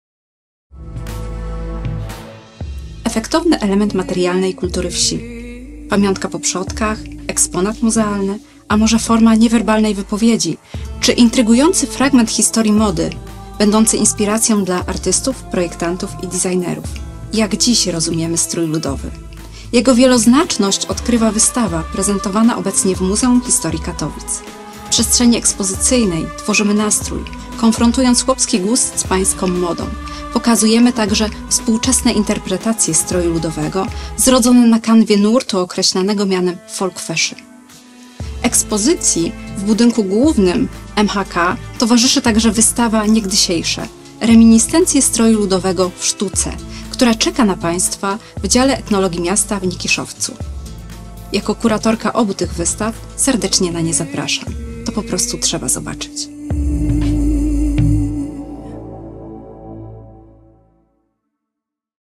Zaproszenie na wystawę „Tworzymy naSTRÓJ. Chłopski gust, pańska moda i folk fashion”
Zaproszenie na wystawę „Tworzymy naSTRÓJ. Chłopski gust, pańska moda i folk fashion”  prezentowaną w Muzeum Historii Katowic (budynek główny, ul Szafranka 9) oraz na wystawę „Niegdysiejsze…Reminiscencje stroju ludowego w sztuce”, którą można zobaczyć w oddziale MHK -Dziale Etnologii Miasta na Nikiszowcu.

realizacja, montaż: Ireneusz Gajdosz